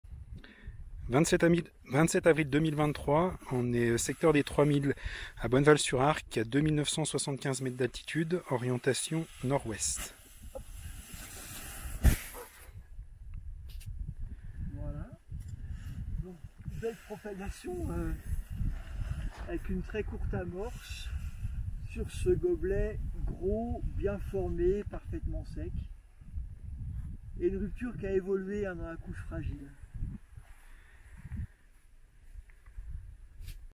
2023-04-27. Bonneval sur Arc. Altitude 2975 m, Exposition NW. Test nivologique.
PST positif. Au moins en profondeur, couche fragile de gobelets restée sèche, épaisse et potentiellement active. Explique peut être des avalanches inattendues signalées récemment, notamment dans les Hautes Alpes voisines.